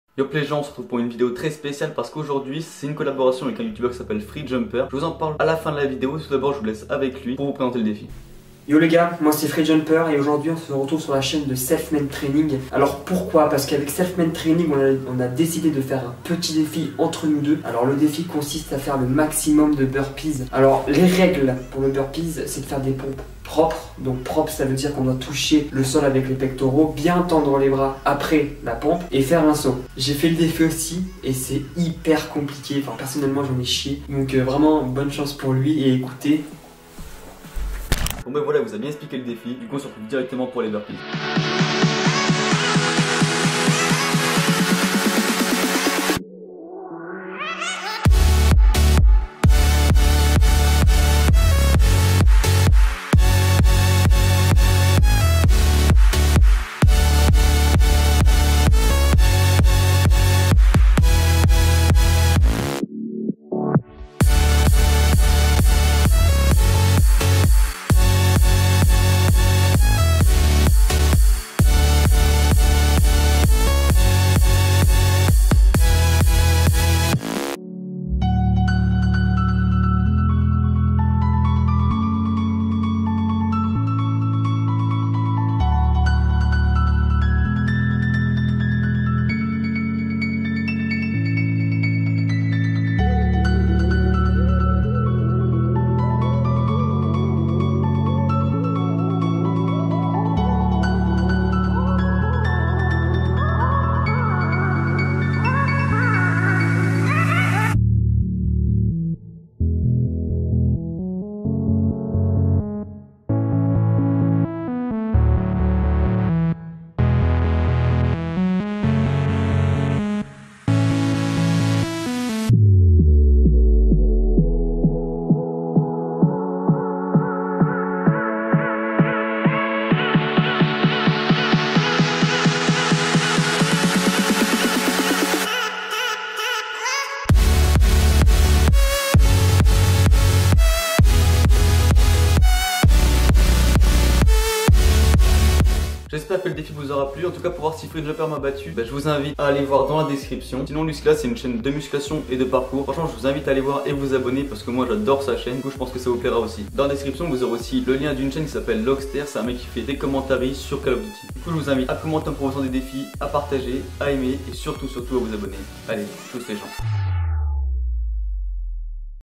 MAX DE BURPEES SANS REPOS (feat. Fr33 Jump3rs)
Un YouTuber me lance un défi ! Réaliser le plus de burpees sans prendre de repos !

Hugo
SelfMadeTraining